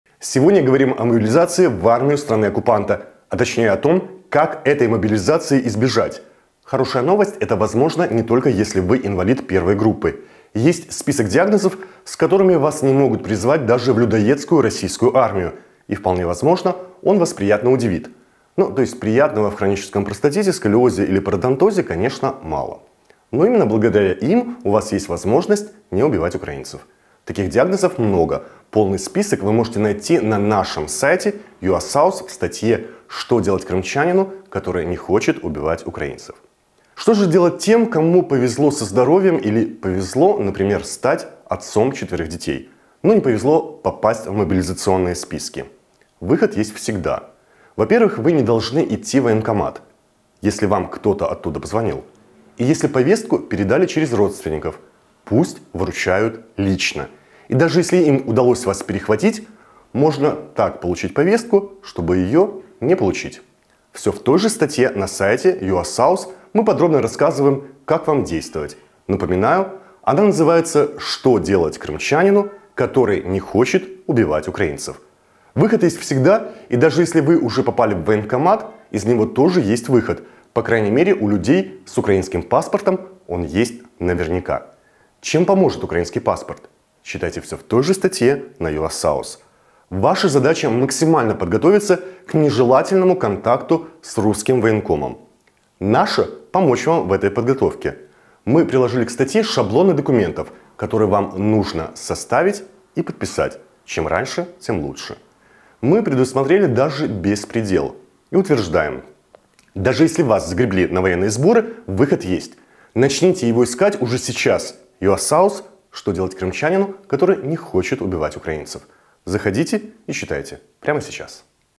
ЧТО ДЕЛАТЬ, ЧТОБЫ НЕ МОБИЛИЗОВАЛИ В РФ. КРЫМ
Мы собрали всю необходимую информацию, которая может помочь в уклонении от службы в российской армии. Шаблоны заявлений и болезни, которые освобождают от воинской службы.

Заходи и читай! Прямо сейчас!

Читайте наші відповіді на найважливіші для вас питання на нашому сайті